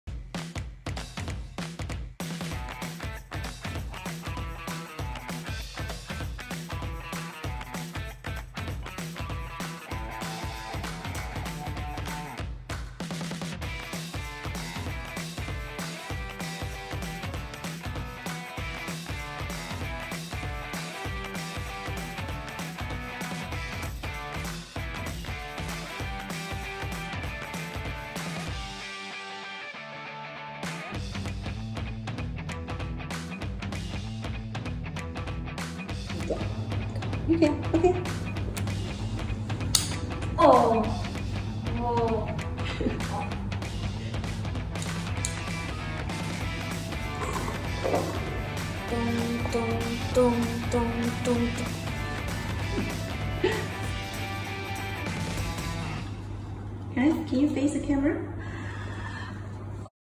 Abstract Ice Cream Painting by kid using Palette knife - Art kids painting tutorial
Time lapse video of Anna Grace ( 6 years old )  and her ice cream painting ! Using palette knife .